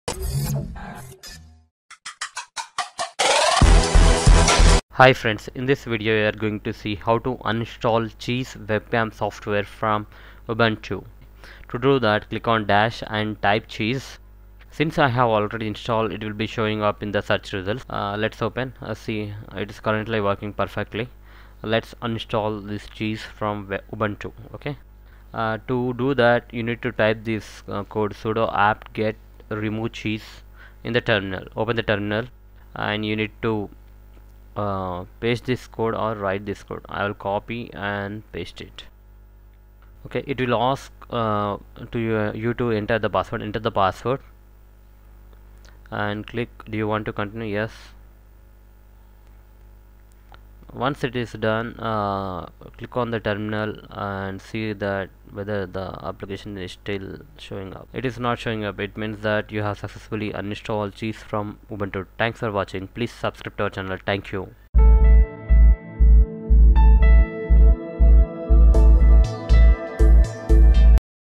Uninstall Cheese Webcam Software application from Ubuntu 14.04 | Linux
Steps to remove Cheese :-
1) Open the terminal
2) Type / paste the below code
     sudo apt-get remove cheese
3) Enter the password & follow the instructions to remove
That's it you have succesfully removed Cheese webcam software from ubuntu operating system